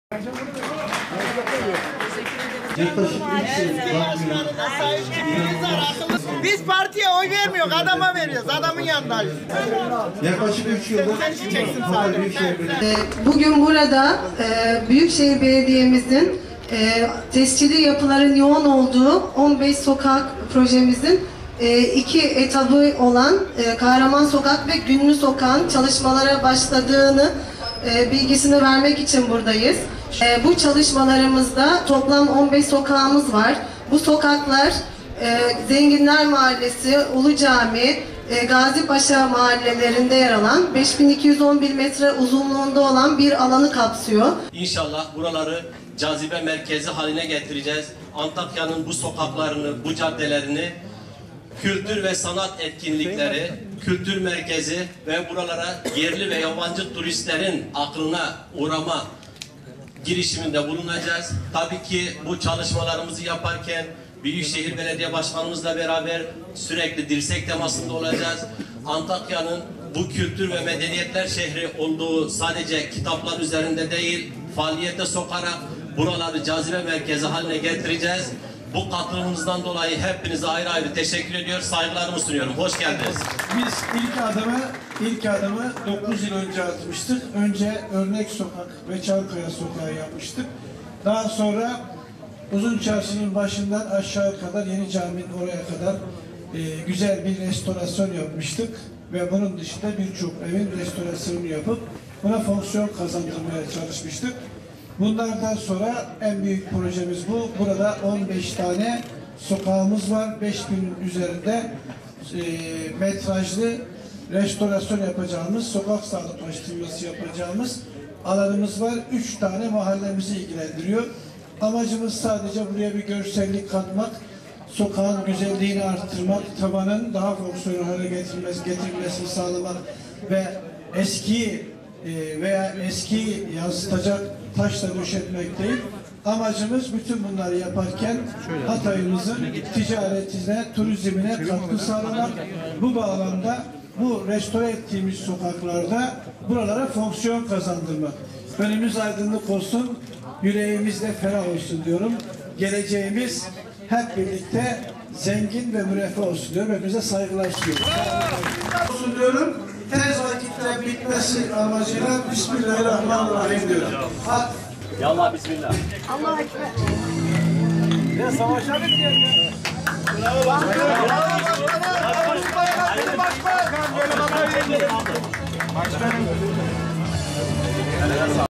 TARİH KOKAN SOKAKLARA BÜYÜKŞEHİR ELİ DEĞİYOR
Hatay Büyükşehir Belediyesi(HBB) tarafından Antakya’nın Ulu Cami, Güllü Bahçe, Zenginler, Yeni Cami, Şehitler ve Gazipaşa mahallerinde bulunan bazı eski sokakların restorasyon çalışmalarına başlandı.
Toplam uzunluğu 5 bin 500 metre olan 15 cadde, sokak ve çıkmazlarda gerçekleştirilen iyileştirme ve yenileme çalışmalarının açılışına Hatay Büyükşehir Belediye Başkanı Doç. Dr. Lütfü Savaş da katıldı.
Antakya’nın tarih kokan eski sokaklarında gerçekleştirilen açılışta Başkan Savaş’a CHP Hatay Milletvekili Mehmet Güzelmansur, Millet İttifakı Antakya Belediye Başkan Adayı Hikmet Hatunoğlu, HATSU Genel Müdürü Mehmet Çaparali, siyasi parti temsilcileri,  STK ve oda temsilcileri ile çok sayıda mahalle sakini eşlik etti.
Sokak ve caddelerde yapılan çalışmalarla ilgili bilgi veren KUDEB Şube Müdürü Ratibe Buğrahan, “Gerçekleştirilen çalışmada sokağa bakan cephelerde çatı onarımı, sıva-boya tamiratı, eski ve özgün taşların ortaya çıkarılması ve aydınlatma yapılıyor. Çalışmalarımıza Kahraman Sokak’tan başladık. Belediyemize ait olan 10 tescilli yapımızda da restorasyon çalışmalarına başladık” dedi.
Gerçekleşen açılışta düşüncelerini paylaşan Hikmet Hatunoğlu da projenin Antakya’ya çok yakışacağını ifade etti. Belediye başkanı seçilmesi durumunda bu sokakların Antakya için cazibe merkezi haline geleceğini de belirten Hatunoğlu, bu çalışmayı Büyükşehir Belediye Başkanı Lütfü Savaş ile beraber başaracaklarını belirtti.
Yapılan çalışmaların 9 yıl önce Örnek Sokak’ta başladığını belirterek düşüncelerini paylaşan Başkan Savaş, “Amacımız sadece sokaklarımızın güzel ve estetik görünmesini değil, şehrimizin ticaret ve turizmine de katkı sağlamak. Biz kentimizin güzelliklerimi tüm dünyayla paylaşmak istiyoruz. Asi Nehri’nin güzelleşmesi, EXPO, gastronomi gibi tüm projelerimizi şehrimize değer katması için gerçekleştiriyoruz” diyerek çalışmalarda emeği geçen herkese teşekkürlerini iletti.
Açılıştan sonra sokaklarda gezerek yapılan çalışmaları inceleyen Başkan Savaş, işçilerle birlikte duvar onardı. Daha sonra Hatay büyükşehir Belediyesinin kente kazandırdığı ve açılışının yakında yapılacağı gastronomi evini ziyaret eden Lütfü Savaş, ev hakkında katılımcılara bilgi verdi.